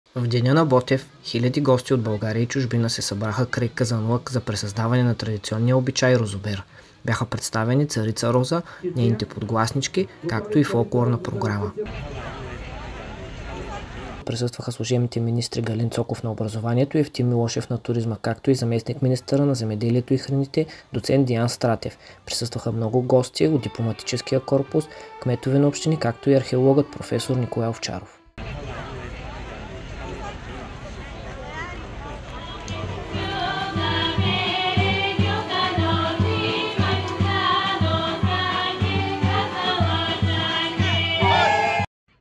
Хиляди гости от България и чужбина присъстваха на ритуала "Розобер" край Казанлък
Хиляди гости от България и чужбина присъстваха на ритуала "Розобер" край Казанлък днес. Сред официалните гости бяха служебните министри на туризма Евтим Милошев и на образованието и науката проф. Галин Цоков, заместник-министърът на земеделието и храните доц. Деян Стратев, народни представители от 49-ото Народно събрание, директори на държавни и общински институции, кметове на общини и на населени места, както и арехологът Николай Овчаров. Гости бяха още посланици и представители на Дипломатическия корпус, както и делегации от побратимени на Казанлък градове.

„Това е един от най-българските празници, който се провежда за 121-ви път. Розата е един от нашите национални символи, а розовото масло е добре познато с качеството си по цял свят", каза зам.-министър Деян Стратев и увери, че производителите имат силна подкрепа в лицето на Министерството на земеделието и храните. "В момента имаме регистрирани 2947 производители, които отглеждат роза Дамасцена и роза Алба, и въпреки последните трудни години, от миналата година насам площите с маслодайна роза са увеличени, като са над 50 000 декара", посочи той и отбеляза, че в България има и 51 производители и преработватели на розов цвят.

Събитието бе съпътствано от фолклорни изпълнения, както и ритуал по представяне на Царица роза и нейните подгласнички.

„Това е нещо изключително, виждаме каква емоция и каква енергия има на Празника на розата. Всичко е много впечатляващо. Днес е и Ден на Ботев, този ден е един голям празник“, каза за БТА Евтим Милошев, служебен министър на туризма.

Според кмета на Казанлък Галина Стоянова почти целият дипломатически корпус е уважил събитието. Стоянова посочи, че историята на розата и розопроизводството се разнася по светаа, а побратимяването на Казанлък с Виетнам разширява границите много надалеч. Кметът очаква потокът от туристи в града да е още по-голям.

Галина Стоянова се обърна към министър Галин Цоков с въпроса за работната ръка, която липсва и дали би помислил за това учениците да помагат в ръчния труд по бране на рози. „Може да се помисли за такъв вариант. Това, което е много важно за нас е тези празници и тези традиции да достигнат до всяко българско дете. С различните инициативи, които имаме в образователните програми, можем да засилим този интерес към доброволчество и те да идват да помагат. Така децата мотивирани от вековната традиция, да се присъединят към вас“, каза министър Цоков.
Репортер и оператор – Радослав Първанов
Монтаж - Ангел Николов